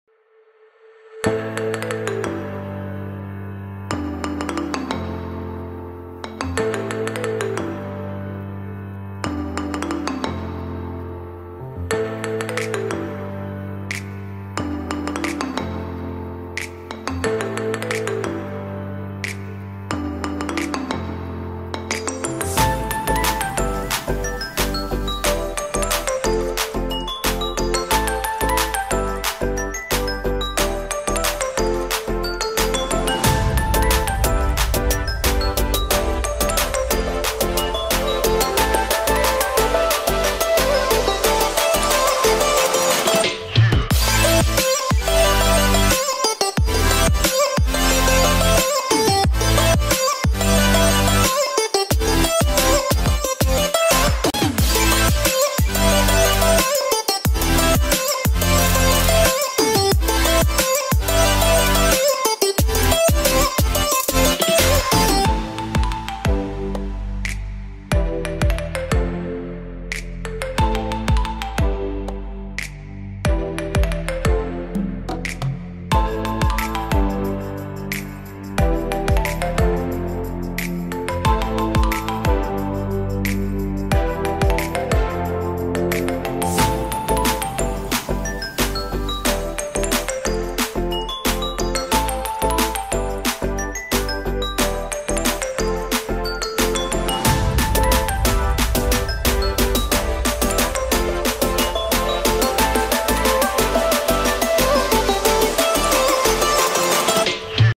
ALL PROMOCODES ON ROBLOX! (That Work)
Today Im Showing You All Promocodes On The Roblox Website That Work So Far!